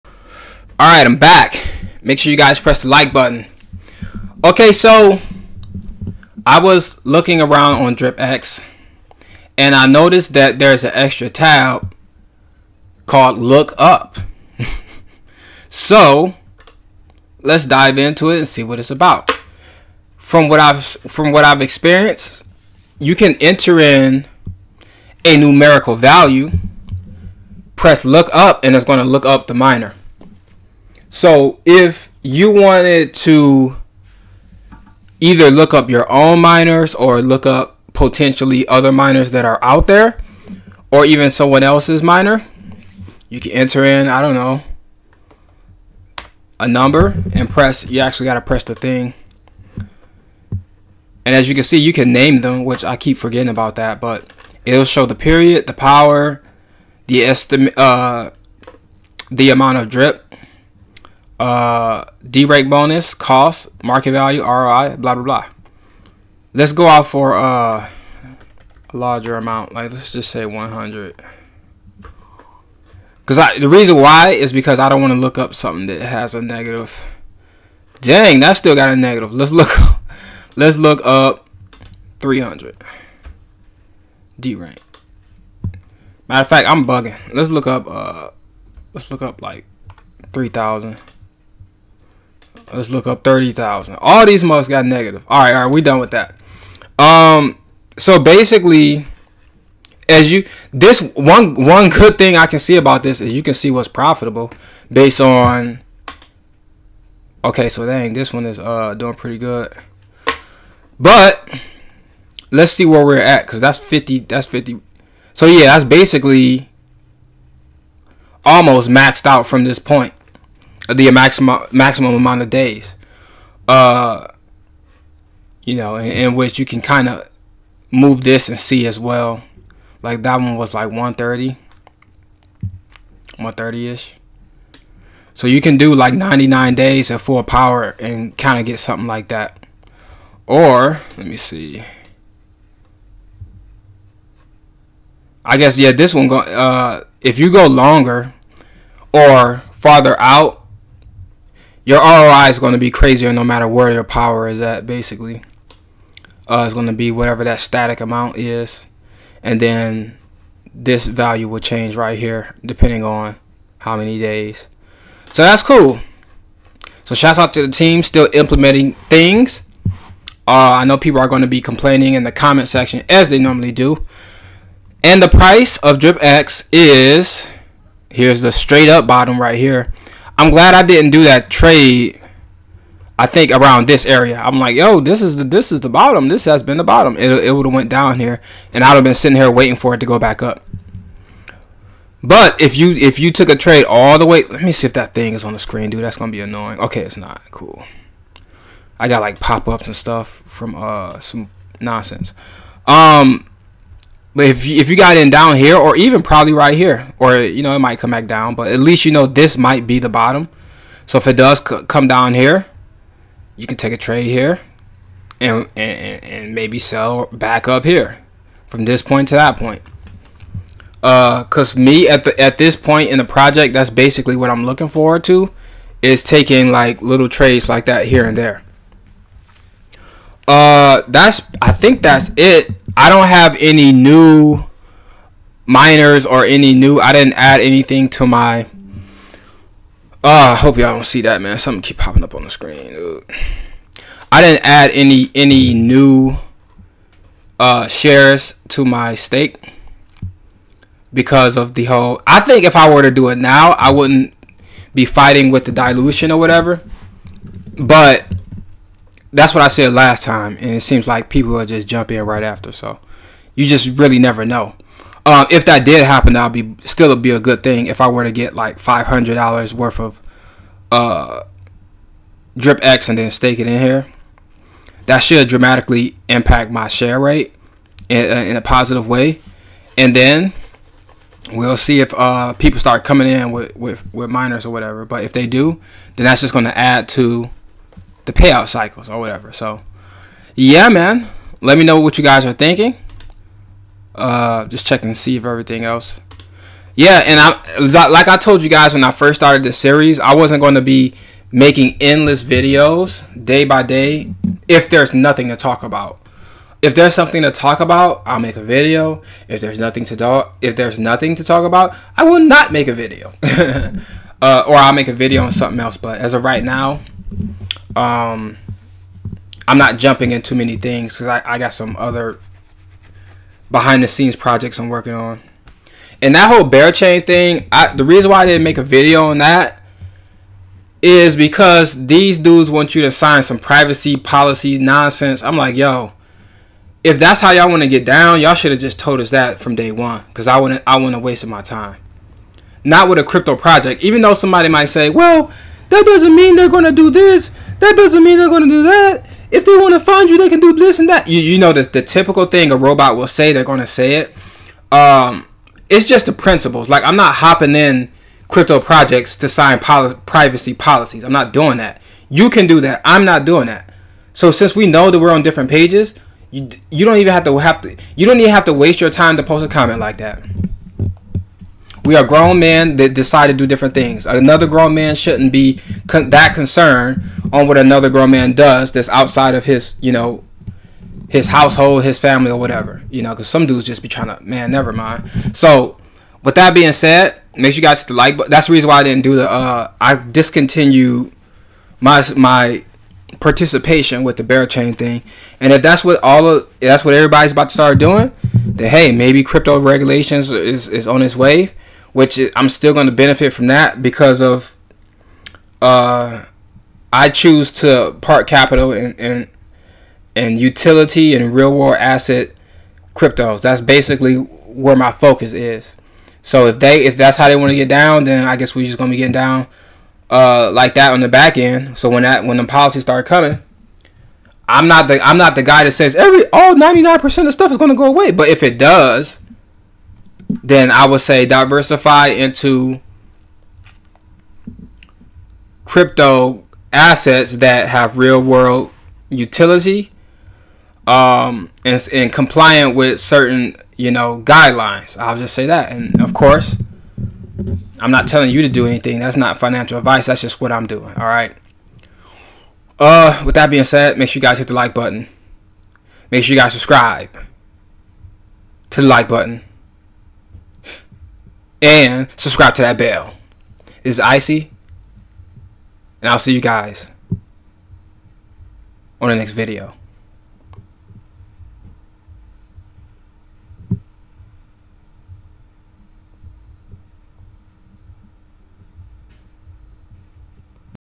Drip Network | DripX New Look Up Feature
DripX is similar to TitanX but not a fork. With the kitchen sink and other products being released, I see this ecosystem going very far.

Will titanx hyper be bullish? I hope so, because it would be great for the overall eco of defi.

This is my dripx strategy and my perspective on how things can play out. Ya mama, for those who diss me.

If you read this, you're awesome!